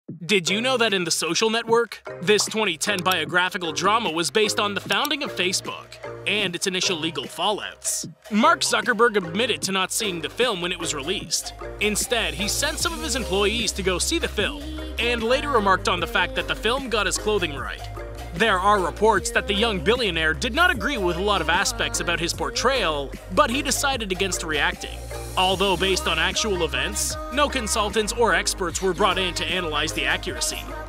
Did You Know That In The Social Network...
This 2010 biographical drama was based on the founding of Facebook & its initial legal fallouts. Mark Zuckerber admitted to not seeing the film when it was released. Instead, he sent some of his employees to go see the film & later remarked on the fact that the film got his clothing right.
There are reports that the young billionaire did not agree with a lot of aspects about his portrayal but he decided against reacting. Although based on actual events, no consultants or experts were brought to analyze the accuracy.